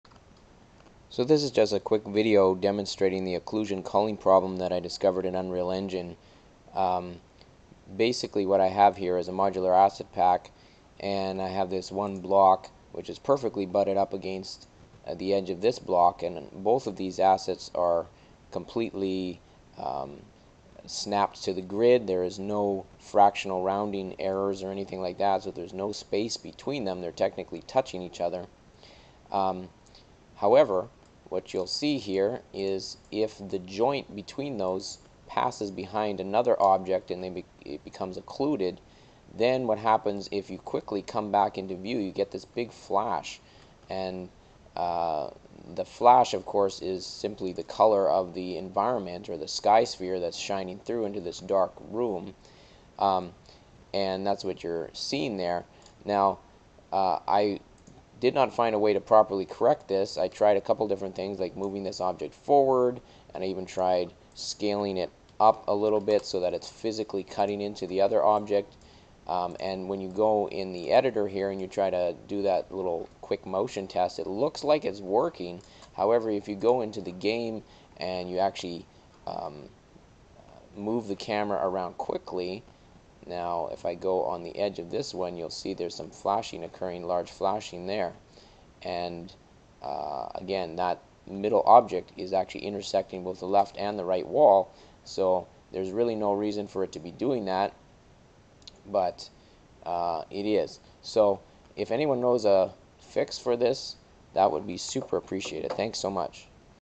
(UE4) White flashes between objects when moving camera quickly
(Looks like an occlusion culling problem. If you move the camera so an object is hidden by another object and then quickly unhidden, you will notice a bright flash where the objects intersect. Does anyone know of a fix for this? I'm using Unreal Engine 4.27.2. Thanks!)